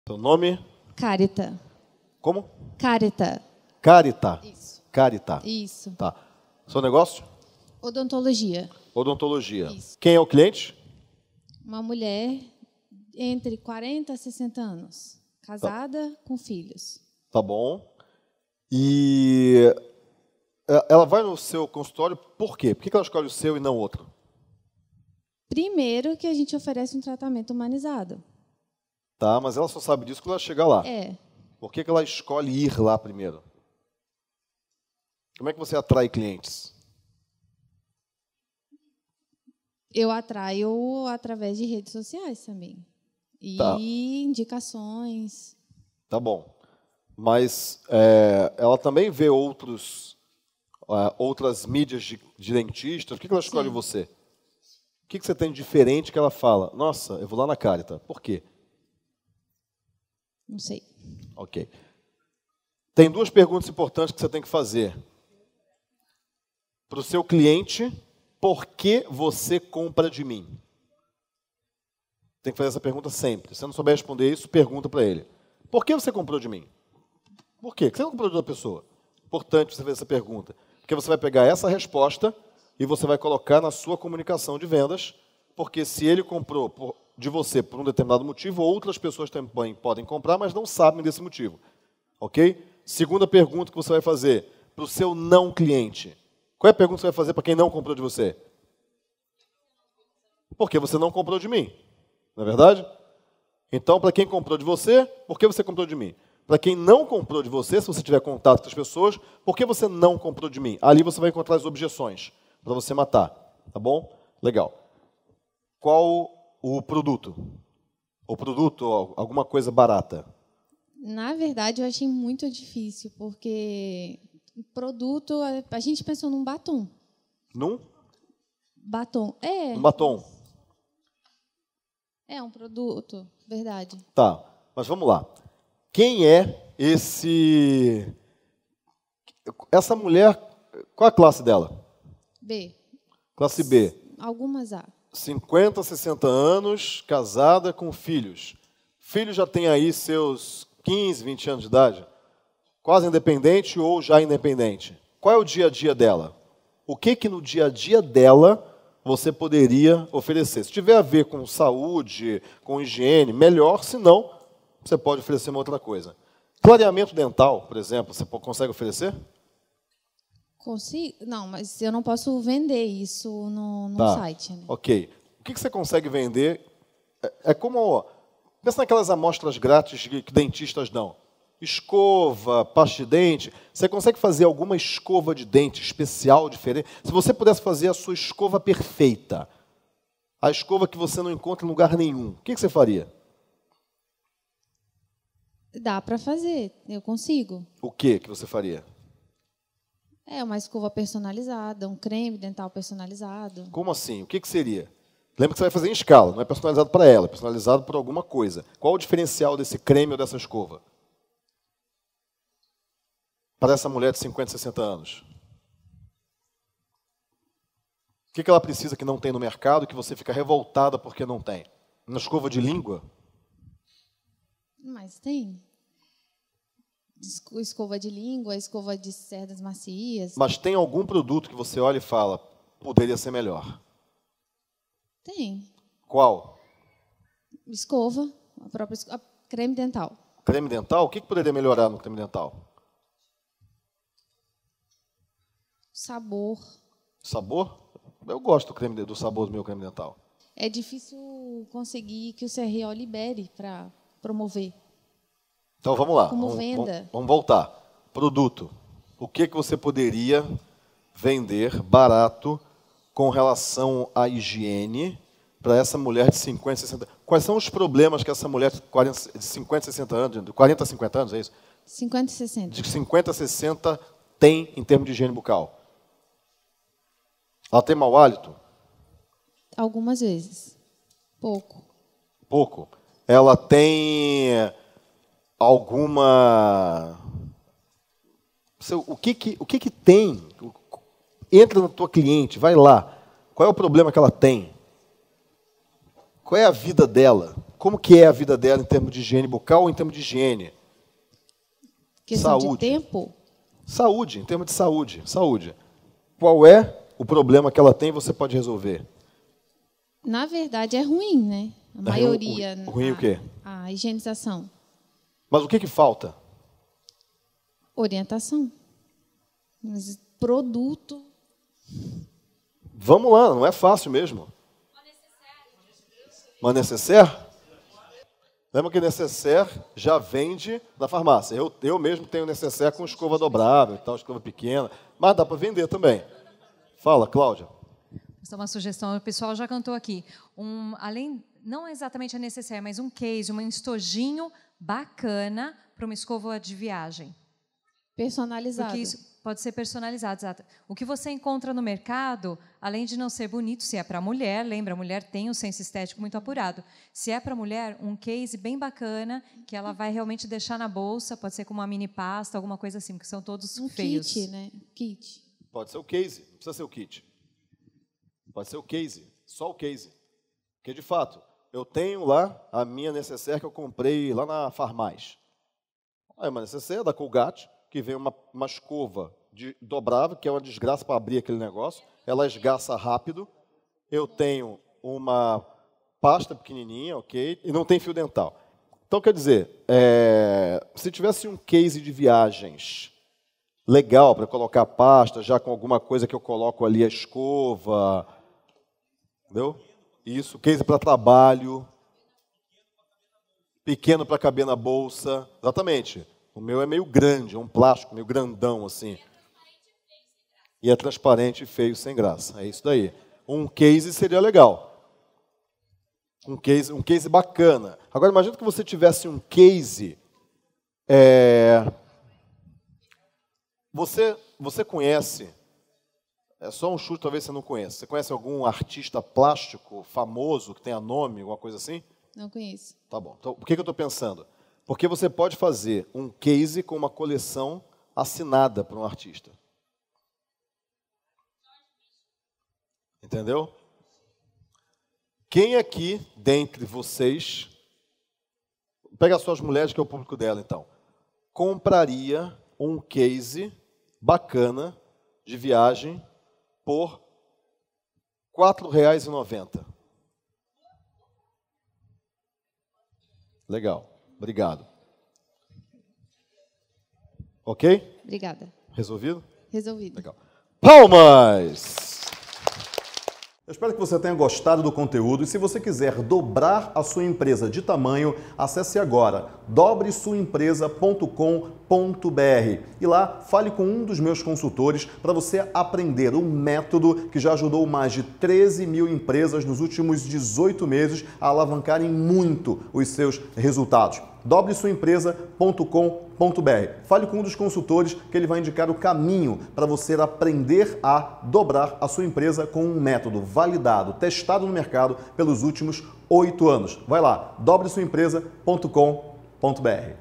INTERVENÇÃO - POR QUE VOCÊ COMPRA DE MIM?
Qual o diferencial que sua empresa oferece? É importante saber o que te diferencia dos concorrentes e usar isso a seu favor.